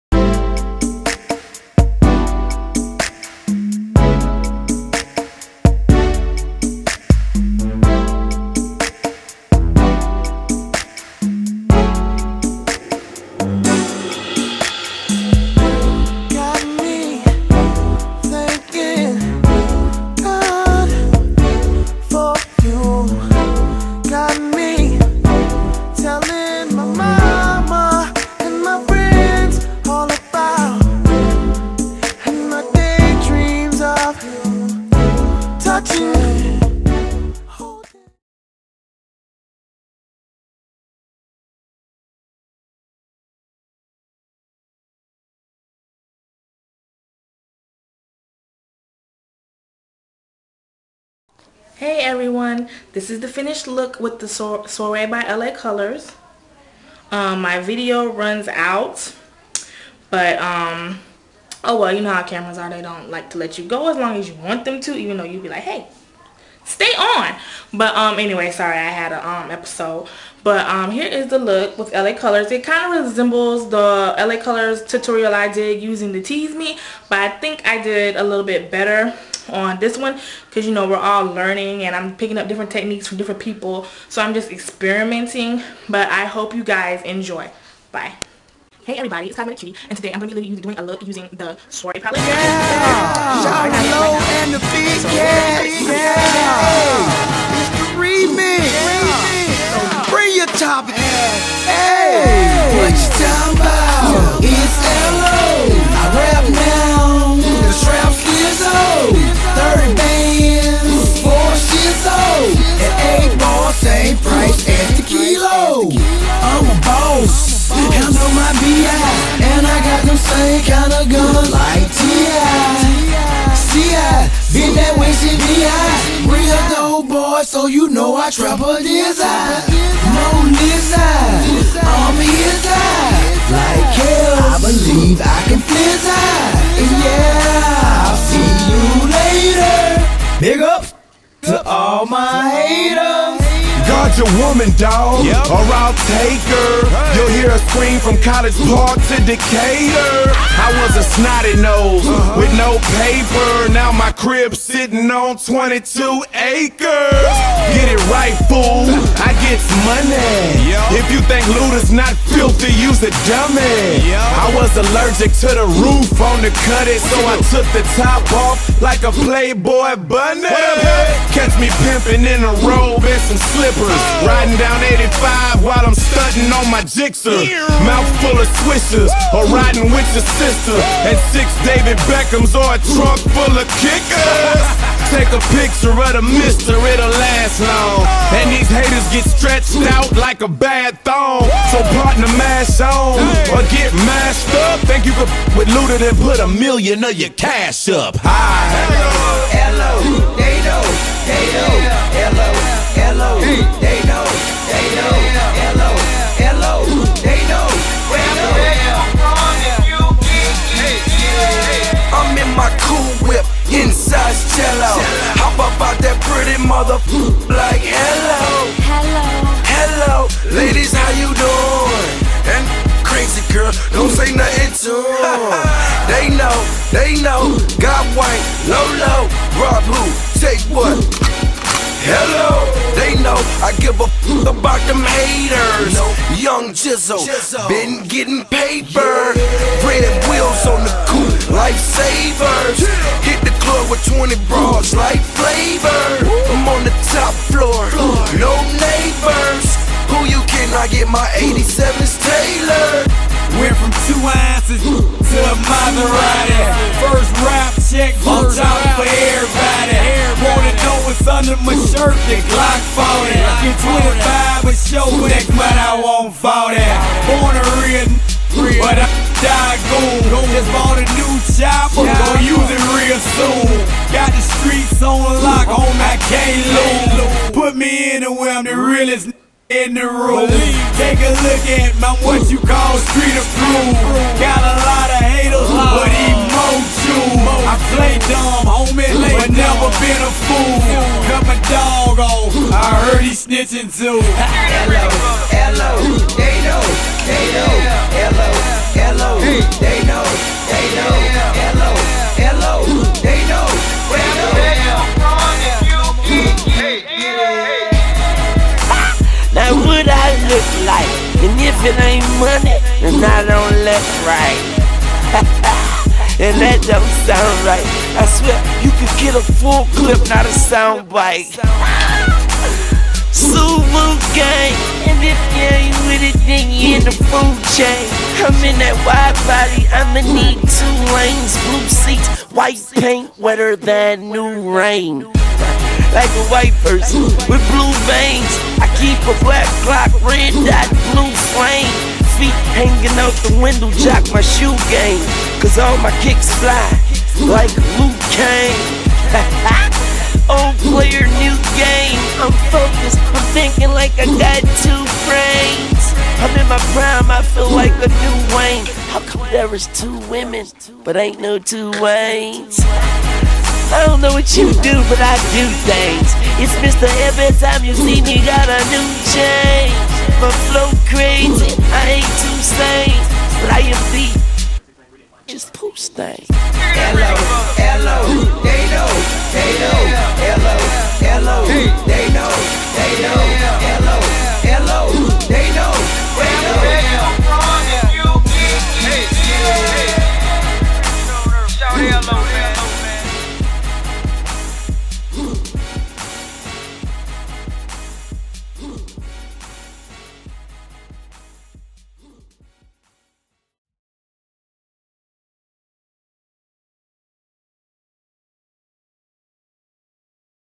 I love Cotton Candy Tutorial
Here is a look I came up with using the LA Colors Palette in Soiree. I think it came out really great.

Products used

- Face Black Radience foundation in Brown Sugar
-La Colors Dual Blusher in Hazelnut

Eyes
-UDPP
- LA COLORS Palette in Soiree
- Milani Color Brillance eyepencil in Aqua
-Loreal Hip Cream eyeliner in black
- NYX Clear Mascara

Lips
-Loreal Hip gloss in Pixie